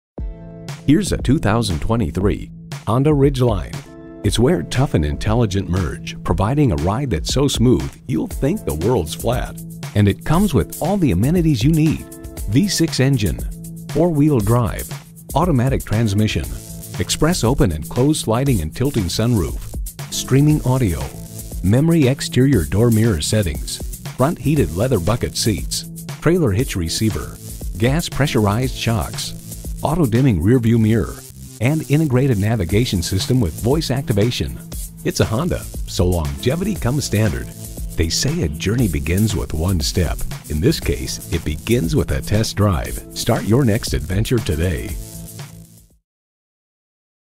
Used 2023 Honda Ridgeline Wausau, WI
Year: 2023
Make: Honda
Model: Ridgeline
Trim: Black Edition
Engine: 3.5L V6 SOHC i-VTEC 24V
Transmission: 9-Speed Automatic
Mileage: 4283
Stock #: 4001FA
VIN: 5FPYK3F86PB022924
Clean CARFAX. CARFAX One-Owner.br2023 Honda Ridgeline Black Edition 4D Crew Cab LOCAL TRADE, Honda Satellite-Linked Navigation System, Navigation system: Honda Satellite-Linked Navigation System. Odometer is 2825 miles below market average! 3.5L V6 SOHC i-VTEC 24V AWD

Address:
2727 North 20th Ave